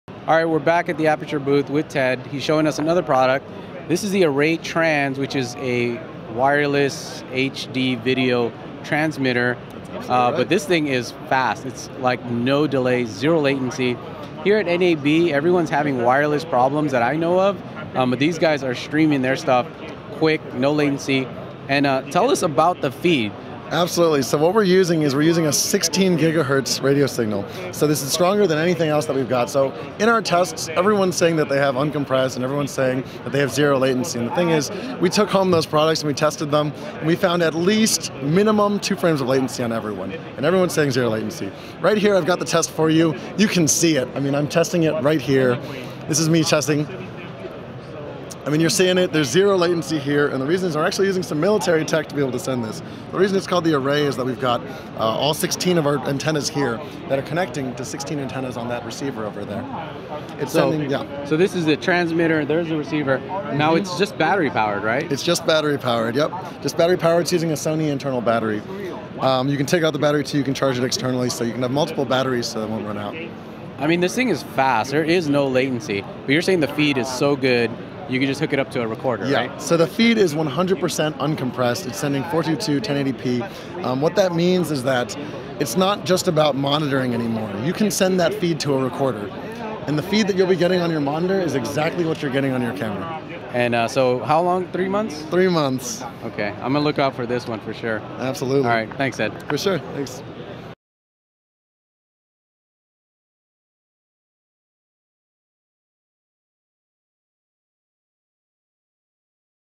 Aputure Array Trans 60GHZ Wireless HD Video Transmitter Receiver
Aputure Array Trans Amazon

This interview was conducted at NAB 2014 about Aputure's new Array Trans 60GHZ Wireless HD Video Transmitter Receiver. The product was delayed on it's release, but is now available.